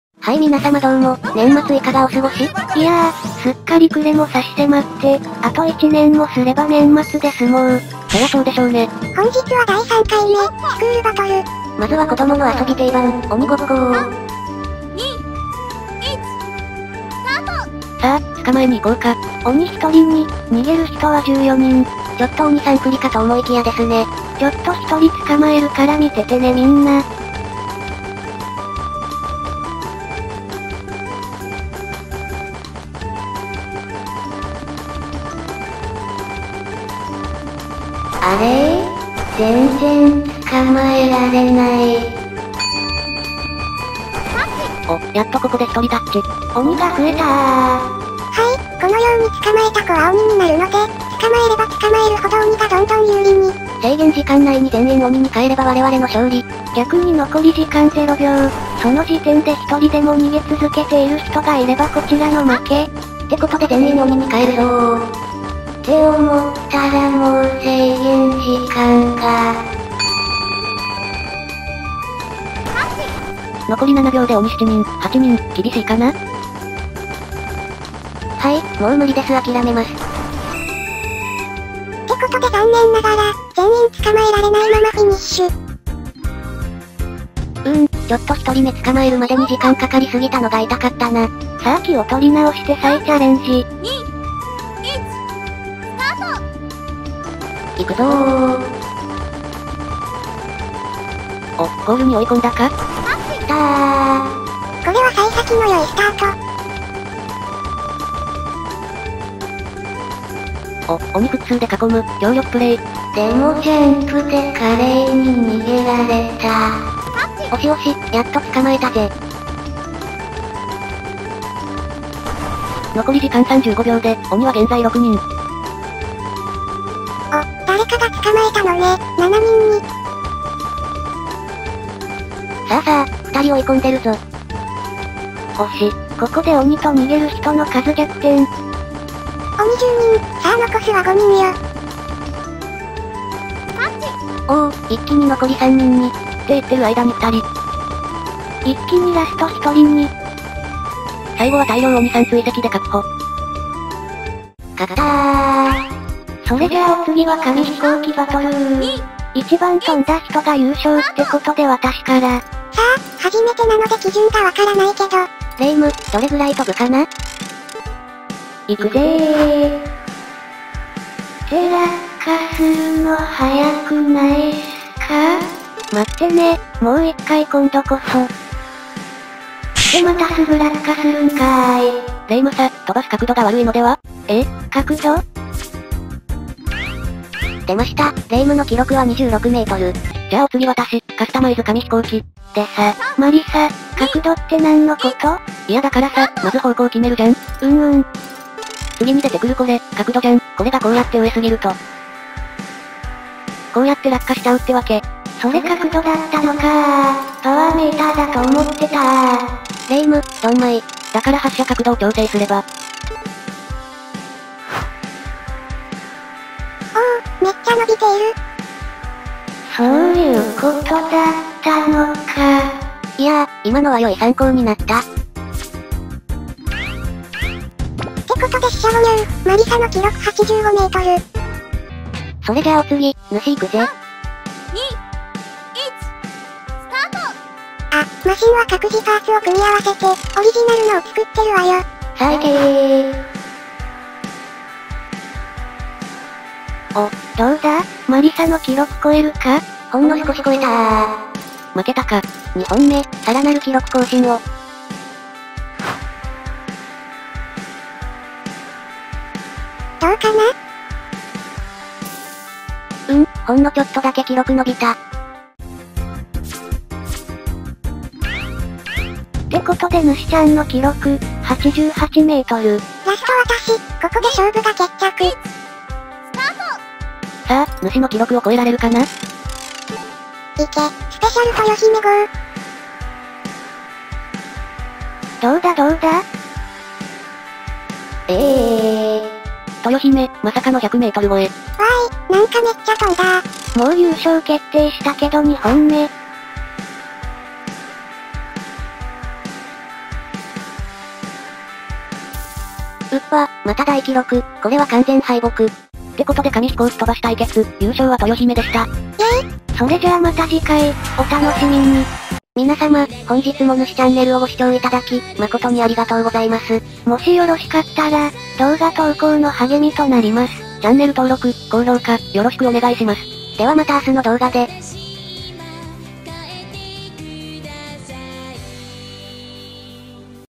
捕まった人はすぐに鬼に変化？！(；ﾟДﾟ)＆紙飛行機バトル！！【スクールバトル#3】
このゲームのよくしていただいてる制作会社様